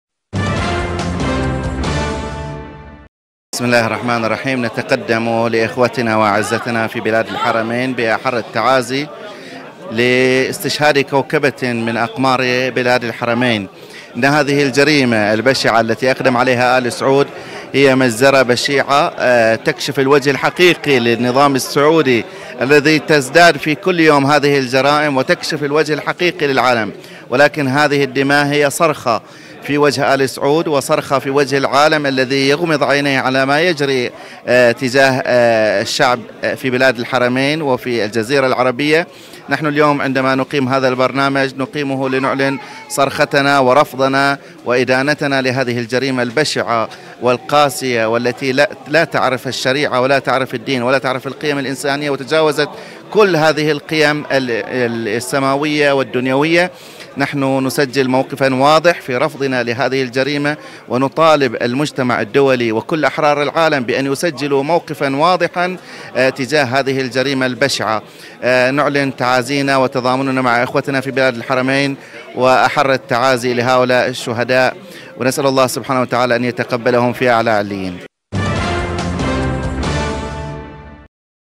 مقابلة مع السيد مرتضى السندي - فعالية صرخة غضب في وجه الشجرة الملعونة 24/4/2019
مقابلة مع القيادي في تيار الوفاء الإسلامي، السيد مرتضى السندي، في فعالية "صرخة غضب في وجه الشجرة المعلونة" التي أقيمت في مدينة قم المقدسة تنديداً بجريمة إعدام آل سعود لـ 33 شخصاً من شيعة القطيف والأحساء والمدينة المنورة